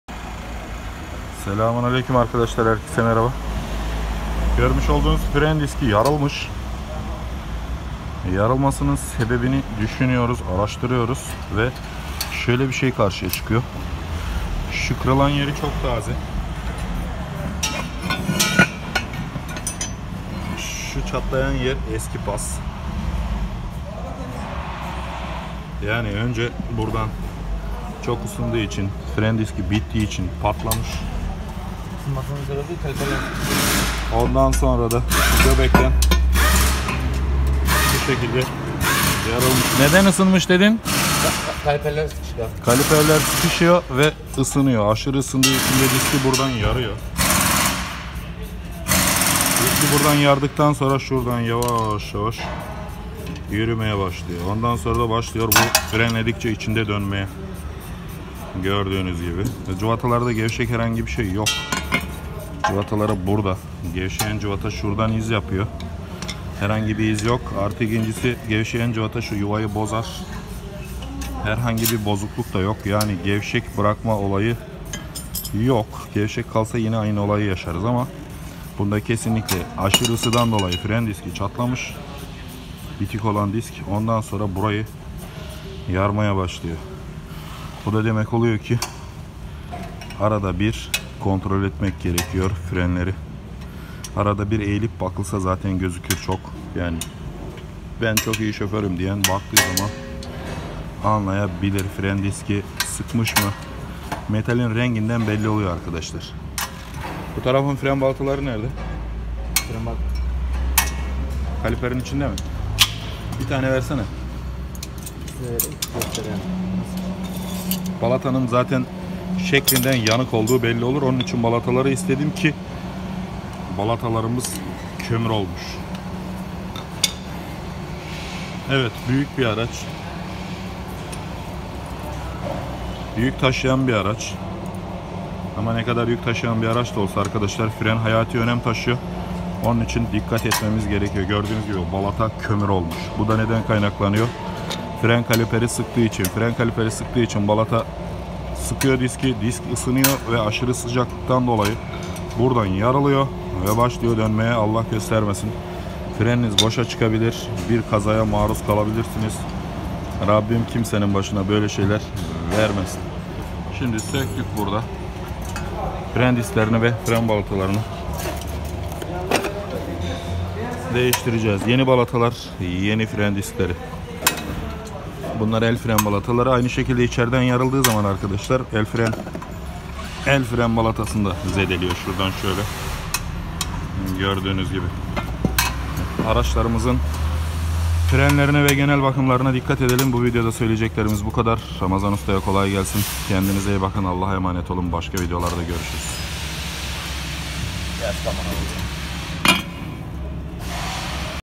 FREN DİSKİ NEDEN YARILMIŞ ( Dikkat hayati tehlike )
Adres: Kepez mahallesi, Hurdacılar caddesi , No 34  Kepez, Antalya   TAMİRHANE

2. Kanalım olan TAMİRHANE VW Bu link'ten ulaşabilirsiniz
bu kanalda sadece vw audi seat skoda marka araçlarla alakalı bilgilere ulaşabilirsiniz